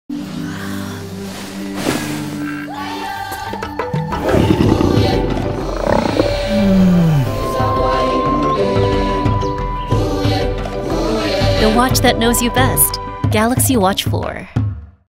Galaxy Watch4：你屬於邊種睡眠動物？
每種動物都代表一種獨特嘅睡眠行為，從而了解更多你嘅睡眠習慣，再幫你計劃並改善睡眠質素。